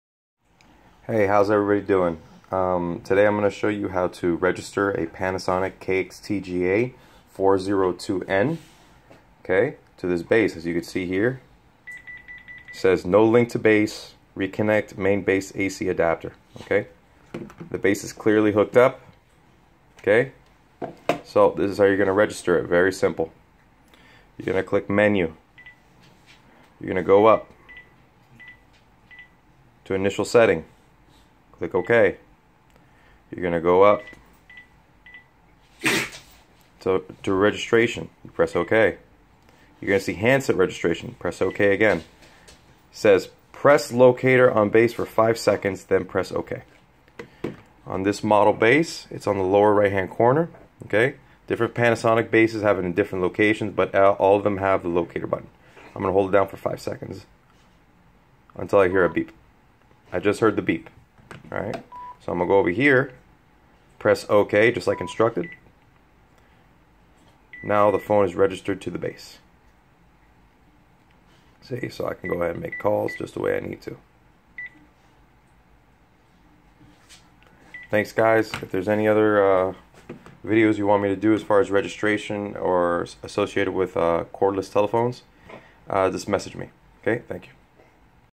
How to register a Panasonic kx-tga402n Cordless Phone Handset
Here I have recorded a video on how to register a Panasonic handset, specifically the kx-tga402 series.